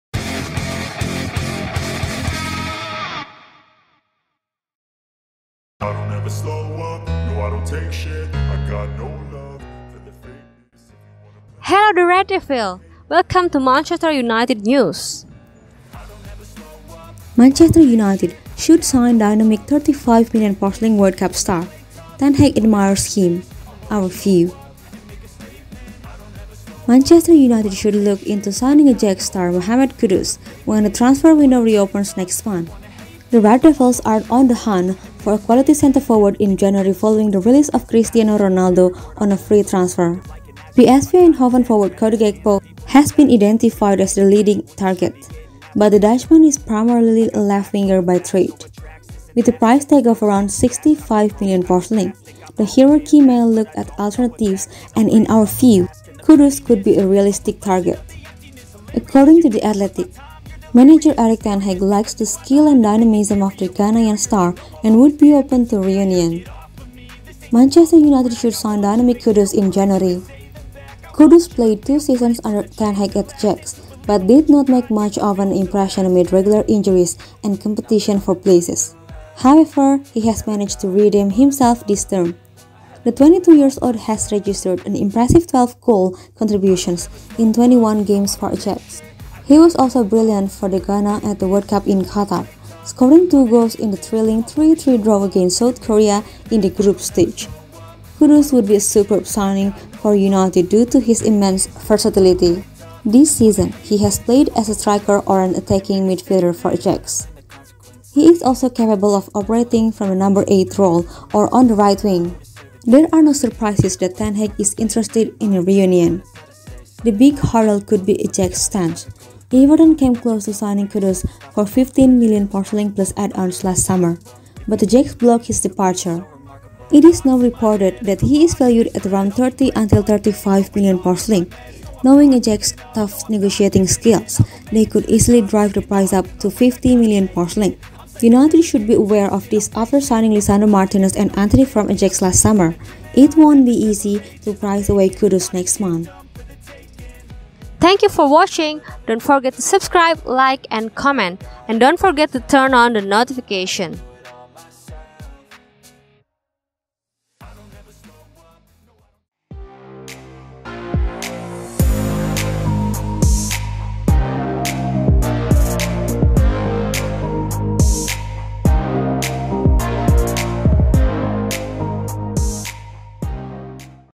ERIK TEN HAG ADMIRES HIM❗ Manchester United should sign dynamic£35m World Cup star
Manchester United should look into signing Ajax star Mohammed Kudus when the transfer window reopens next month.